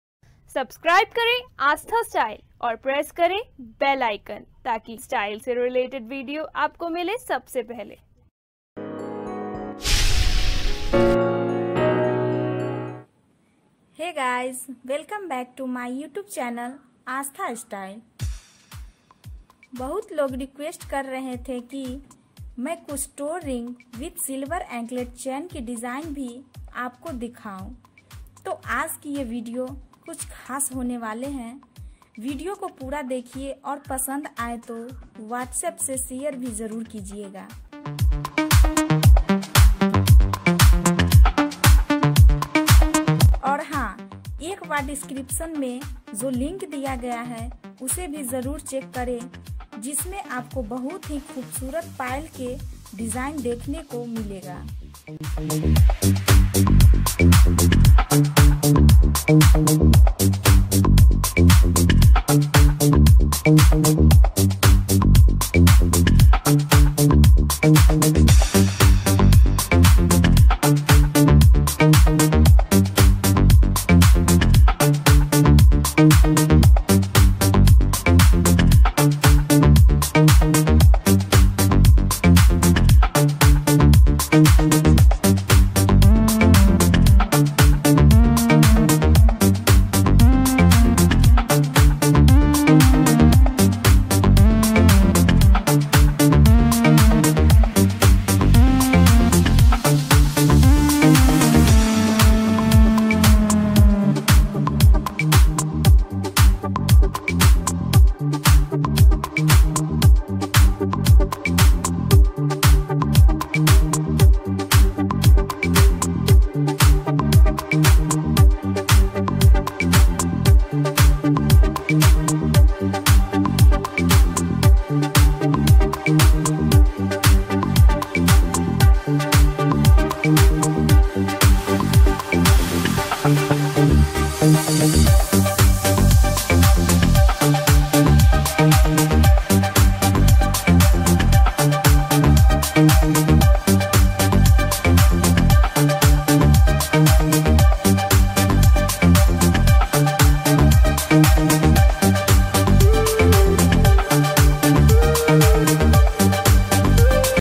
Special Design Toe Ring Chain Silver Anklet | Collection of Chandi Ki Bichhiya OR Payal
Toe Ring with Silver Anklet Chain has been shown in this video. Chandi ke payal bichhiya ke sath. Bichhiya laga huaa dijain.
Bichhiya ke sath payal.